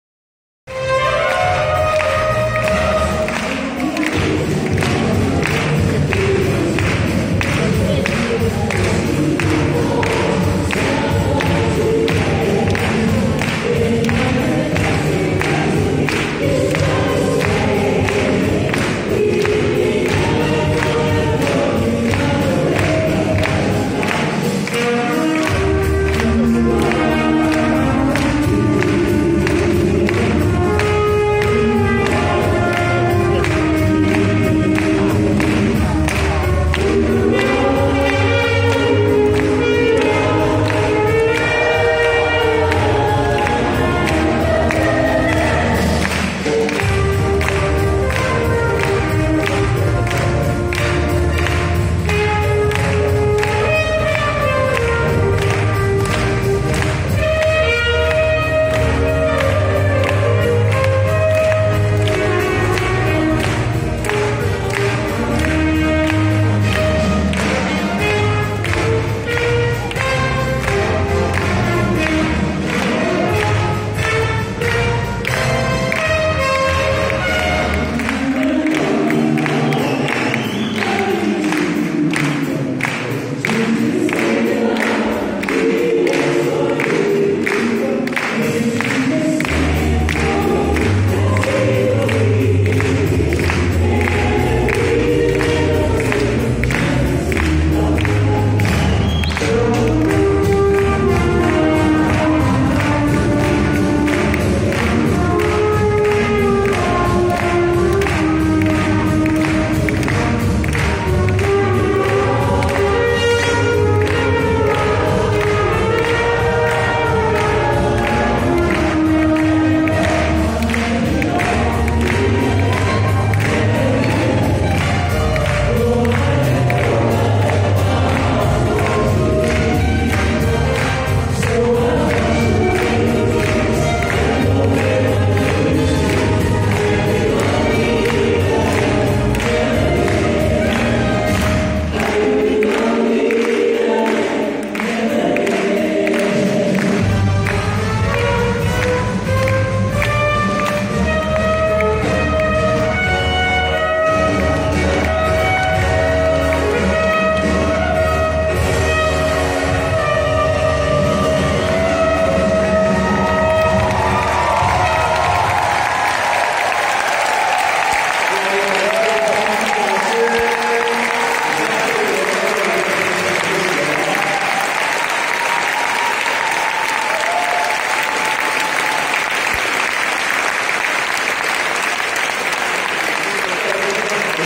彭蒙惠薩克斯風頌讚主- 佳音教會耶誕節主日崇拜 2016/12/25
天韻合唱團表演